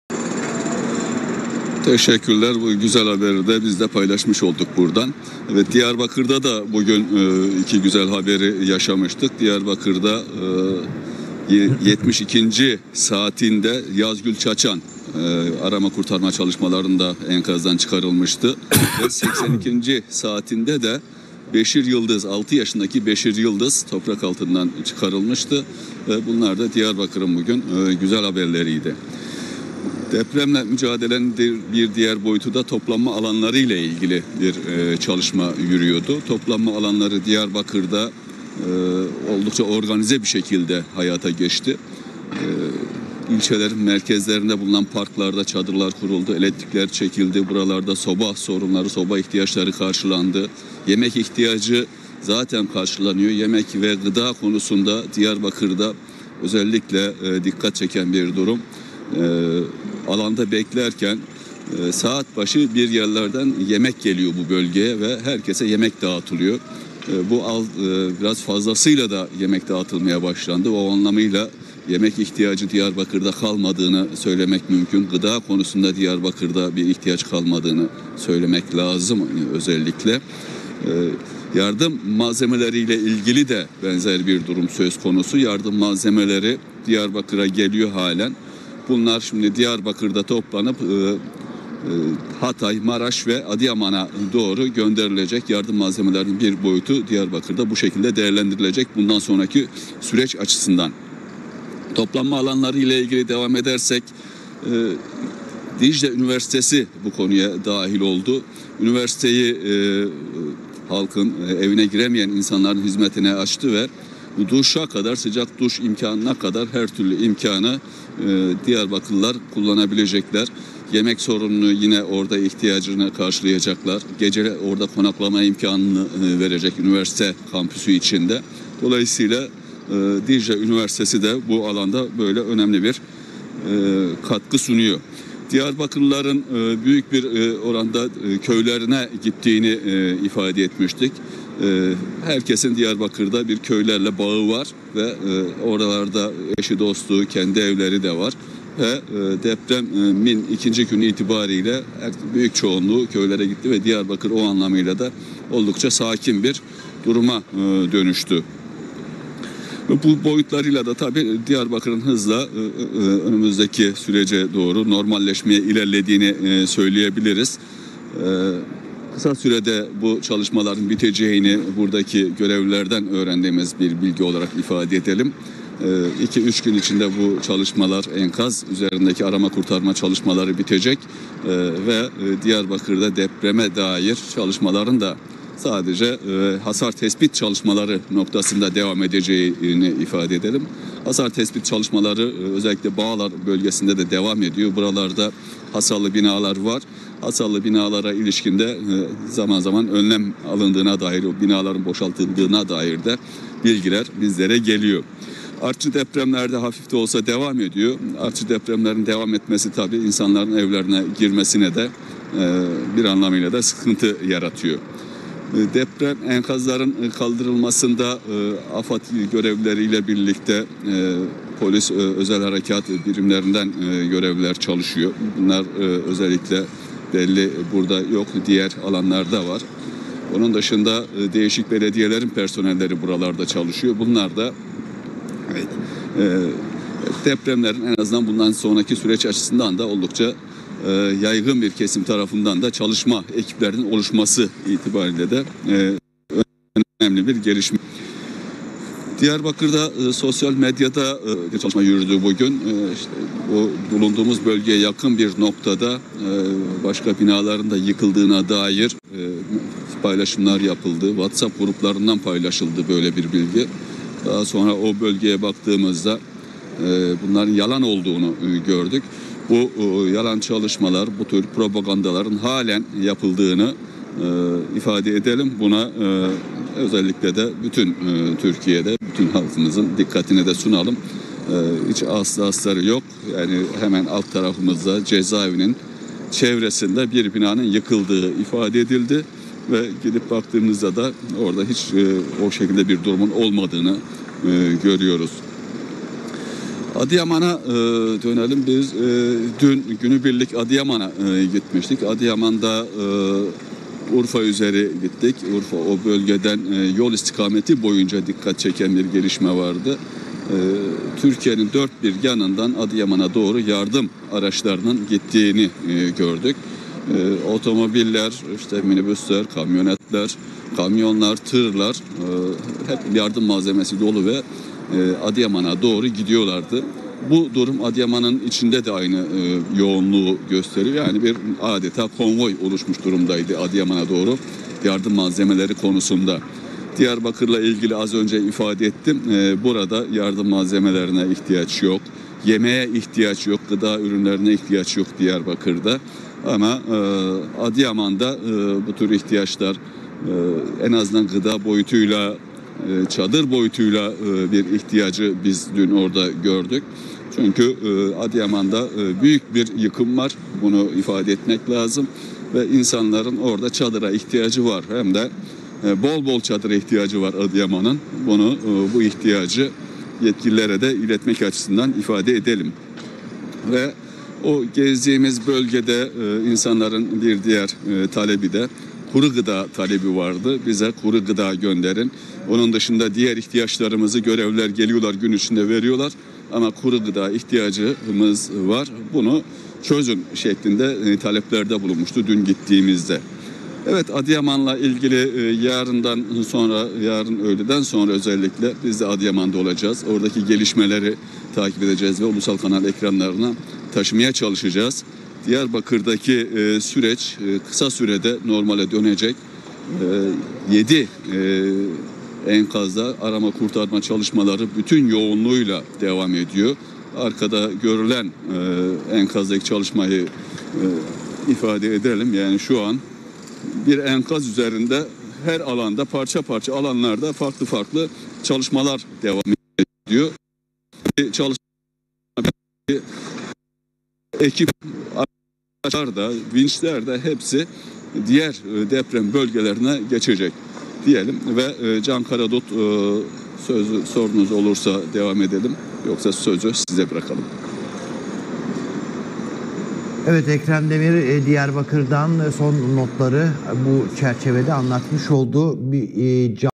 Diyarbakır'dan sevindiren haber | Ekrem Demir bölgeden akardı
Ulusal Kanal Diyarbakır Muhabiri Ekrem Demir Diyarbakır ve Adıyaman'daki son gelişmeleri aktardı. Demir, Diyarbakır'da ihtiyaçlarının büyük çoğunluğunun tamamlandığını ve arama kurtarma çalışmalarında sona yaklaşıldığını kaydetti.